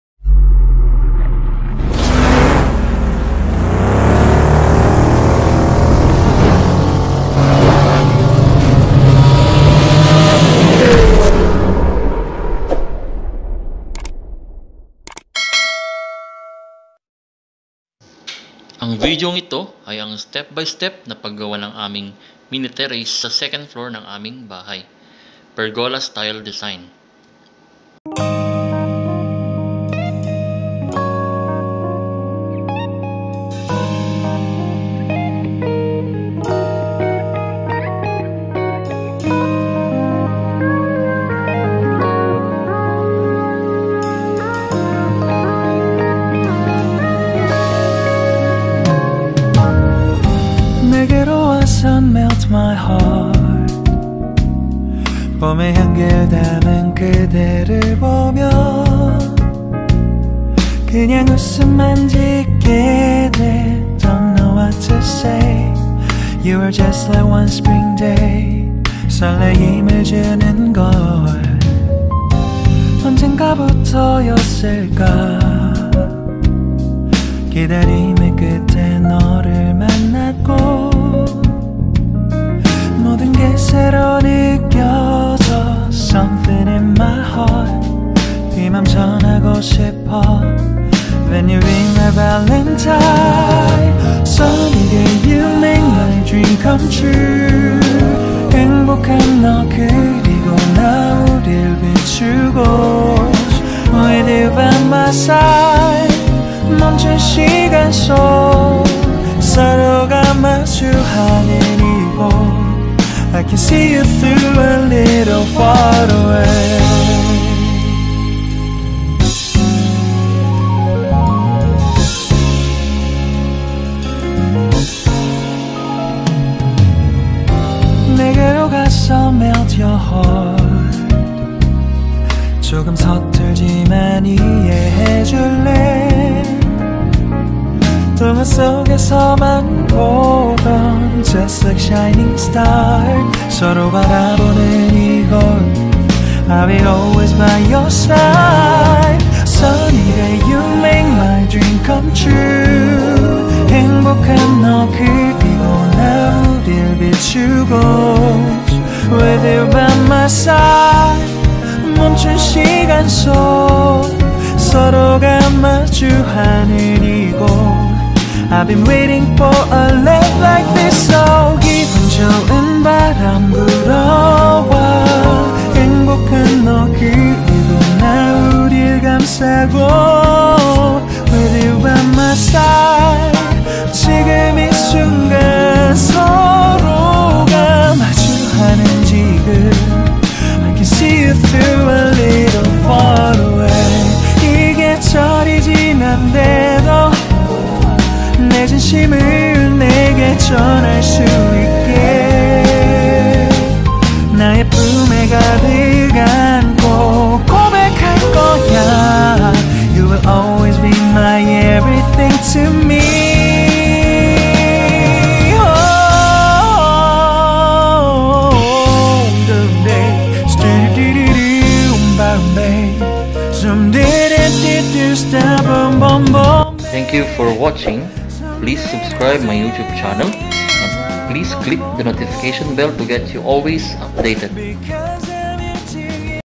KATAS NG OFW|| Our mini Terrace Renovation "Pergola type Design"
Ang videong ito ay ang step step na pag papagawa namin ng aming simple mini terrace sa second floor
ang napili naming design ay "Pergola type design"